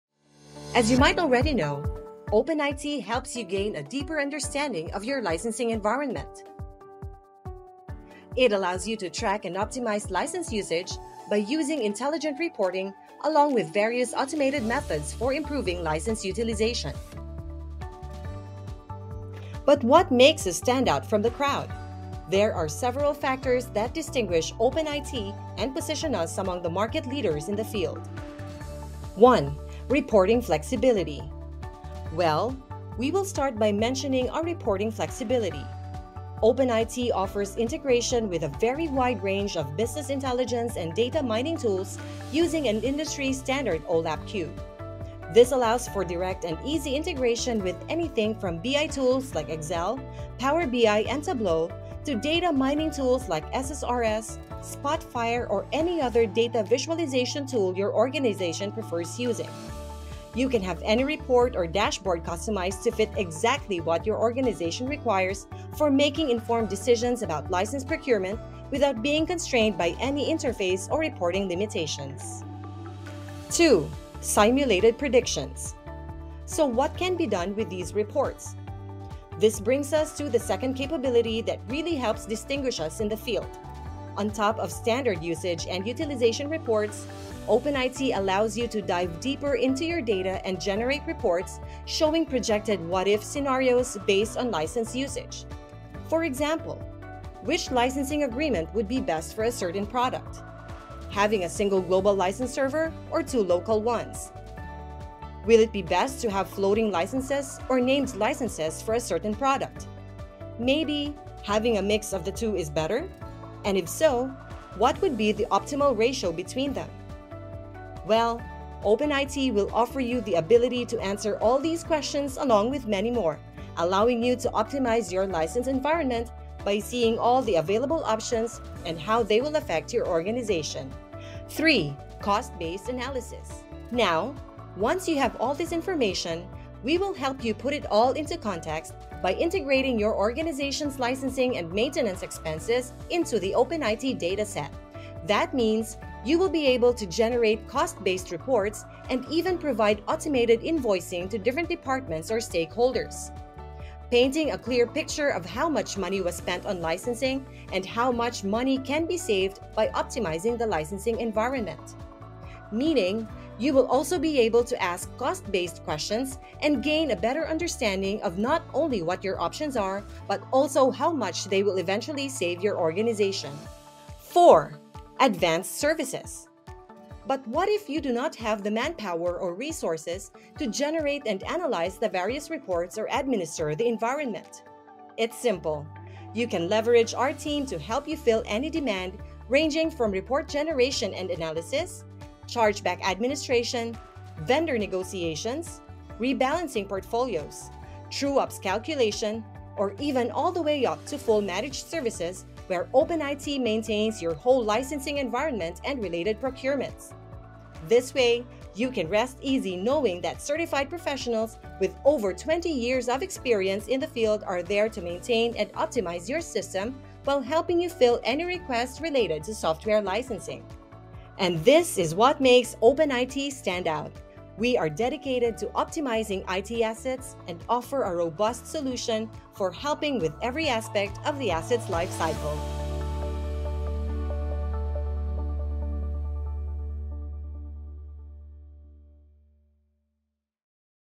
What's In Store When You Use Open iT (Open iT Benefits)
In this video, you will see what Open iT can do for your organization. From reports to dashboards, we've got you covered.